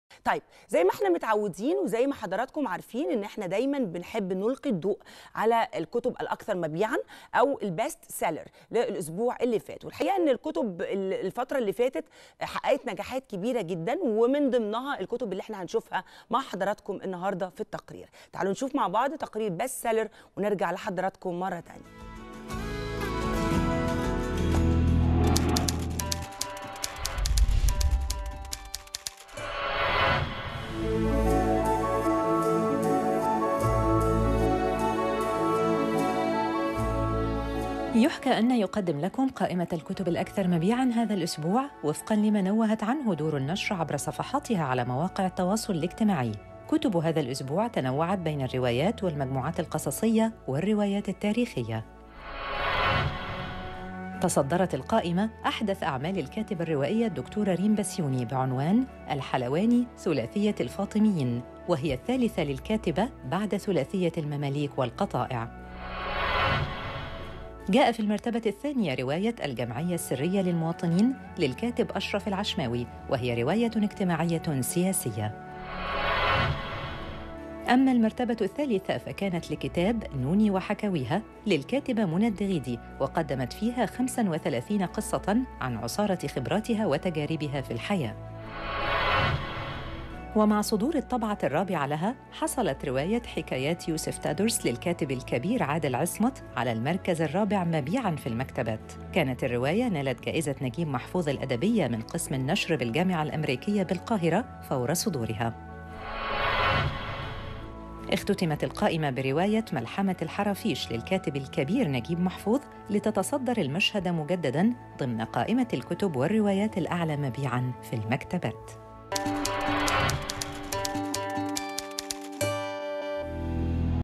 "الحلواني.. ثلاثية الفاطميين" للكاتبة الروائية د. ريم بسيوني تتصدر المبيعات
"الحلواني.. ثلاثية الفاطميين" للكاتبة الروائية د. ريم بسيوني تتصدر المبيعات.. يحكى أن يرصد الكتب الأكثر مبيعاً خلال أسبوع